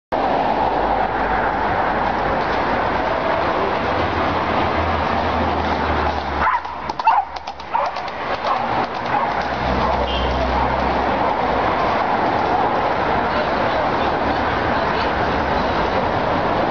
Horse and Dog Running Down Street
In Russia a Horse and Dog randomly running down the street